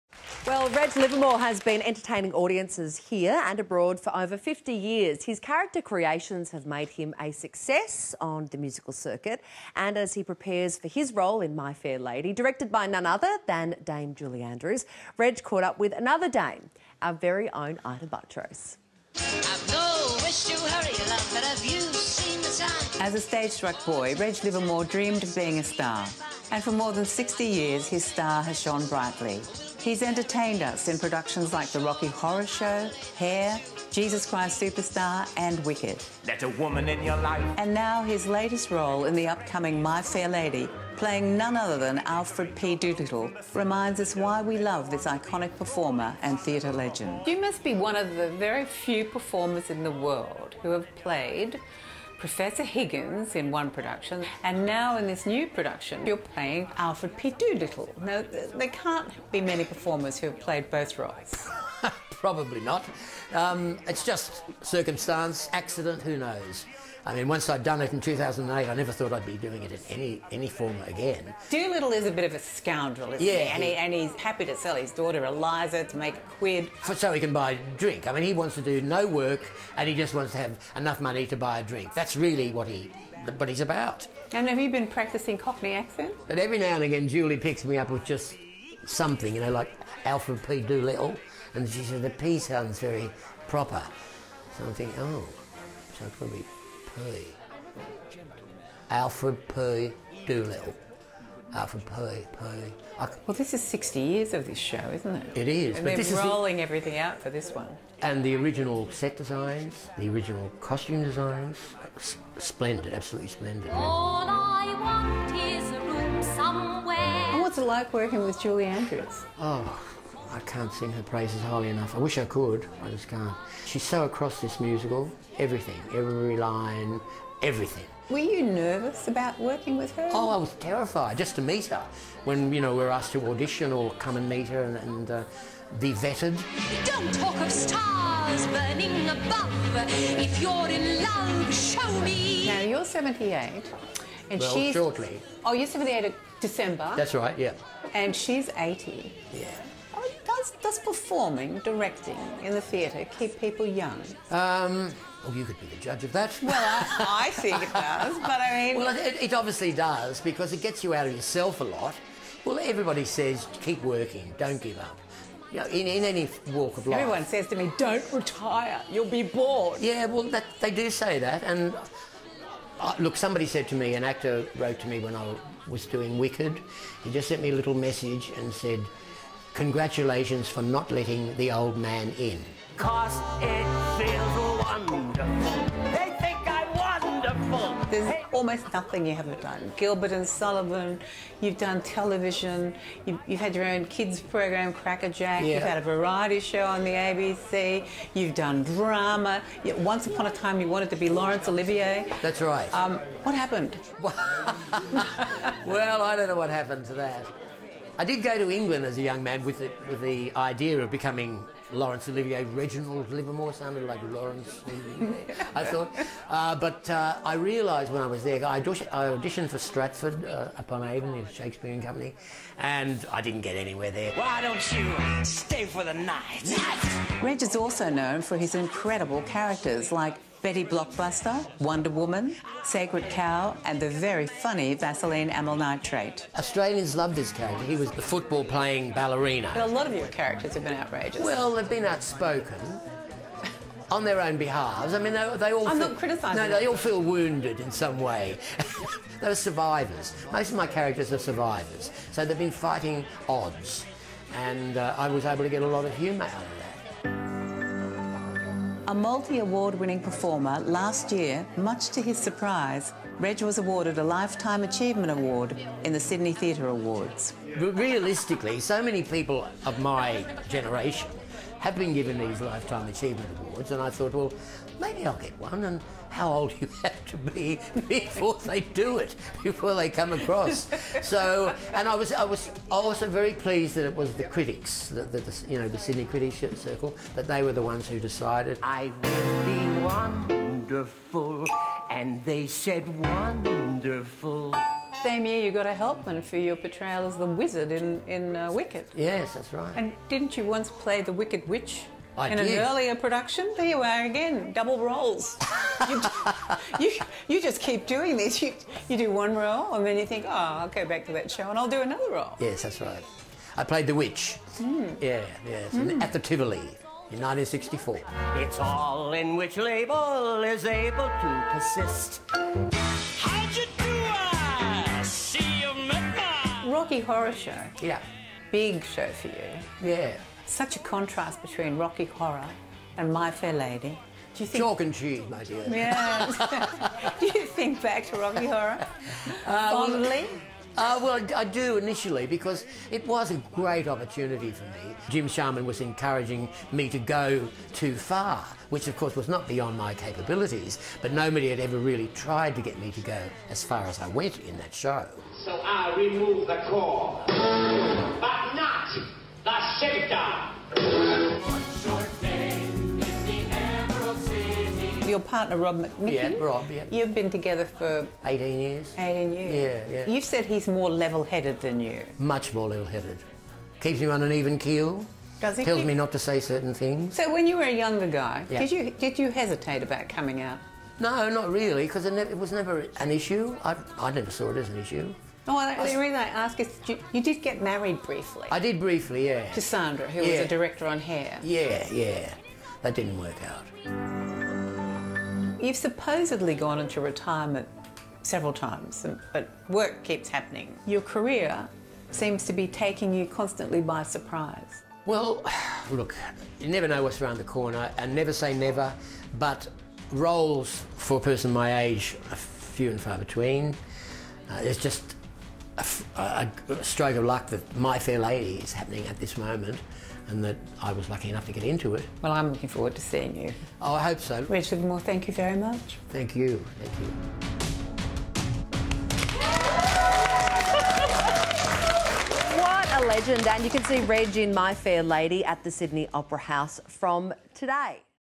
Reg Livermore
Ita Buttrose sat down with theatre great, Reg Livermore.

Join our LIVE studio audience at our studio in Pyrmont in Sydney, Australia.